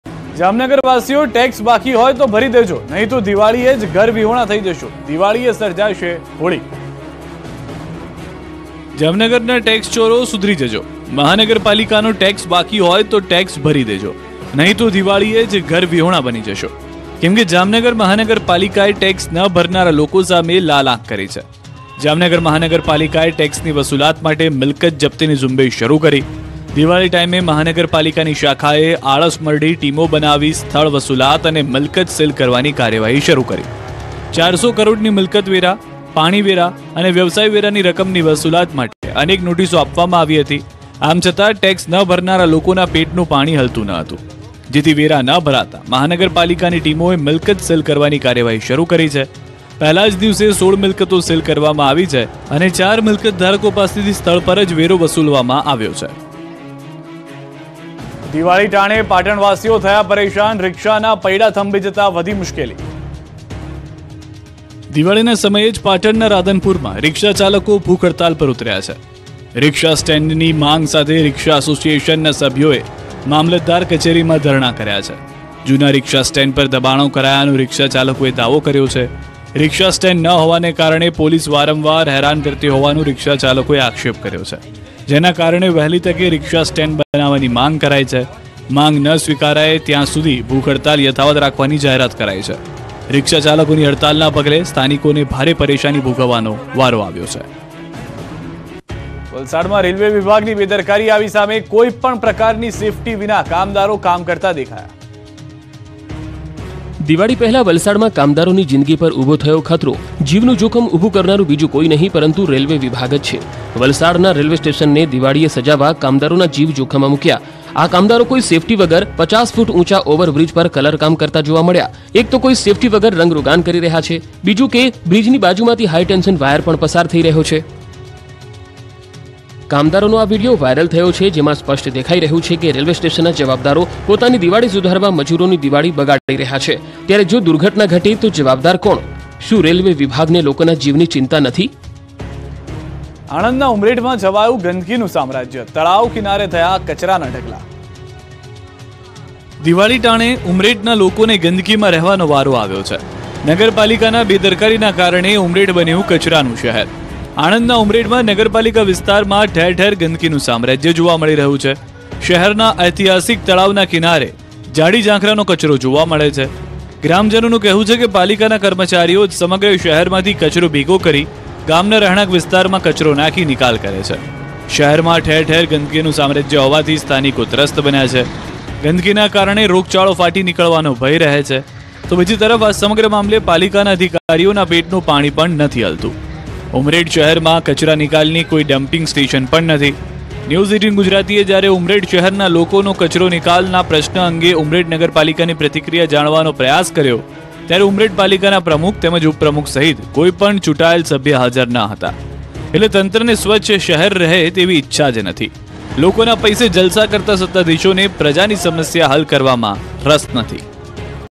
Jamnagar News: ટેક્સ ન ભરનારા સામે તંત્રની તવાઈ | JMC | Gujarati News | News18 Gujarati
Residents of Jamnagar, if the tax is due, please pay it. Otherwise you will be homeless on Diwali itself. Holi will be created on Diwali.

જામનગરવાસીઓ  ટેક્સ બાકી હોય તો ભરી દેજો. નહીં તો દિવાળીએ જ ઘર વિહોણા થઈ જશો. દિવાળીએ સર્જાશે હોળી.